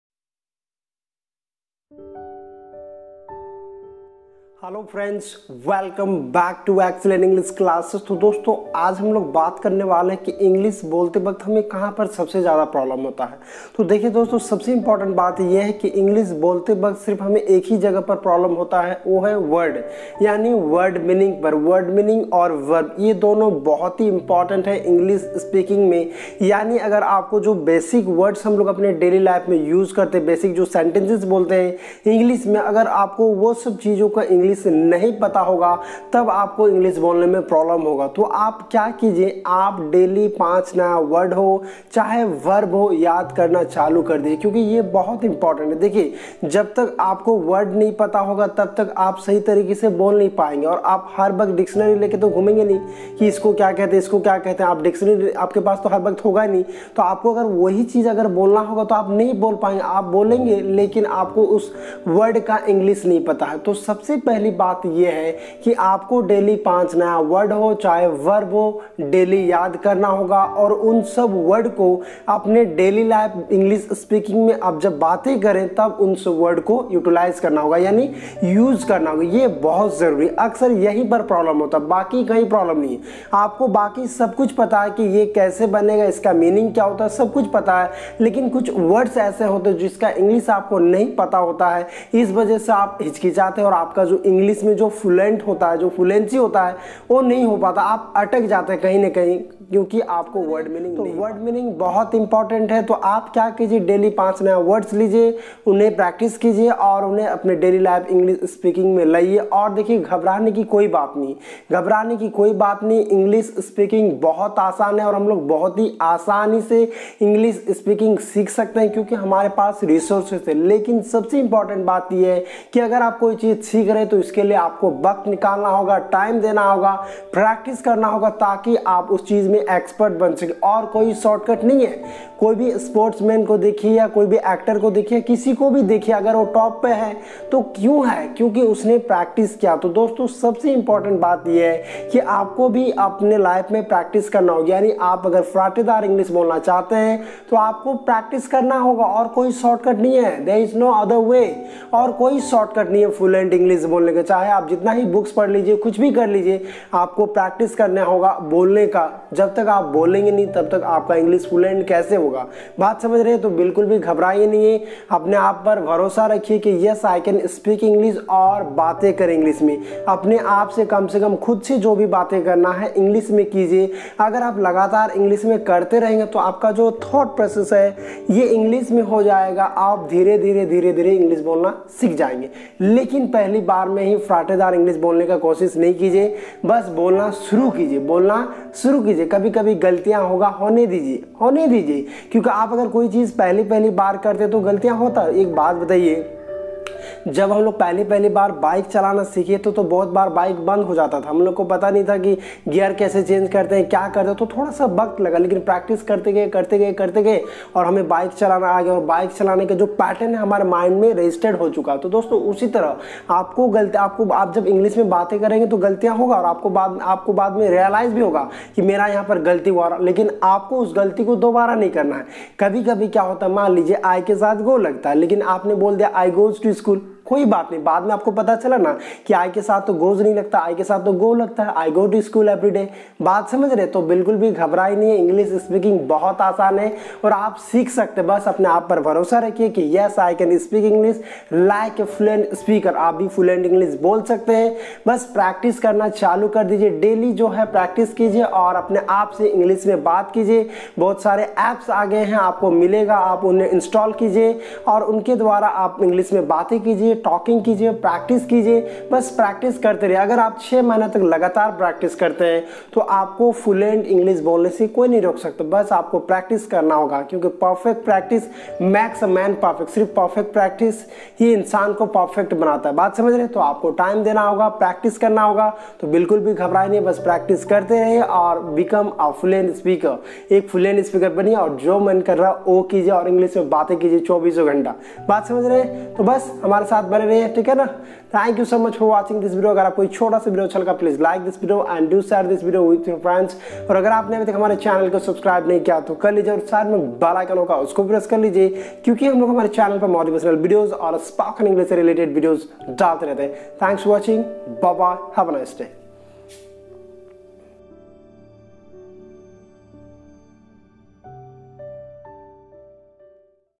बिना अटके English कैसे बोले?|How to speak English fluently|Best tips|By Deepak Sir
Hello friends,is video main maine bataya hai bina atke English mein kaise bat kare aur hame English bolte samay kya problem hota hai pura is video mein bataya gaya hai aap kaise fluently English bol sakte hai wo bhi bina ruke pure confident ke sath bas aapko jo chij bataya gaya hai usko follow karna hai aur apne daily life me apply karna hai agar aap lagatar practice karte rahe to aapko fluent English bolne se koi nhi rok sakta English speaking bahut aasan hai bas apane aap par bharosha rakhiye ki mein bhi fluently English bol sakta hun aur jo bhi aapke dimag mein aa rha hai English mein soche aur khud se English mein bate karte rahiye hamre shubhkamnaye aapke sath hai.